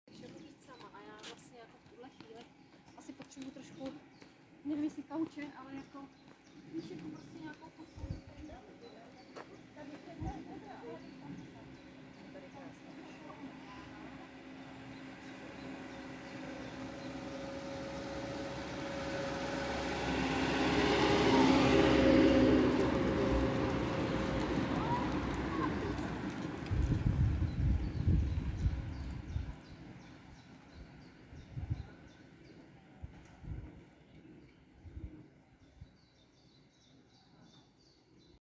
Transport from Frantizcovy Lazne to the zoo.
This tourist transport brings you from Frantizcovy Lazne to the zoo and back to Frantizkovy Lazne. It drives every 30 minutes.